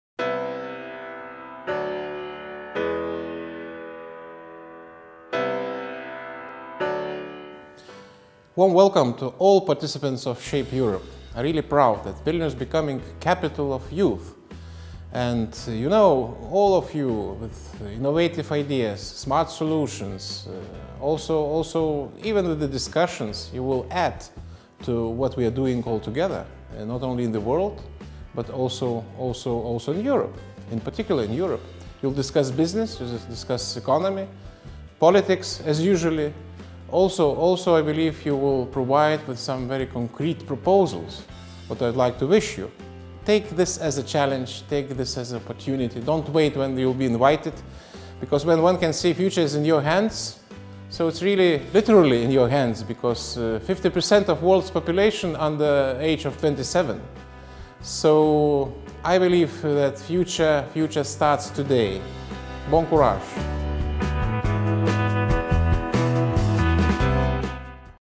Linas Linkevičius greets "Shape Europe" participants
Minister of Foreign Affairs of Lithuania Linas Linkevičius welcomes WEF Global Shapers and other guests to the annual meeting "Shape Europe".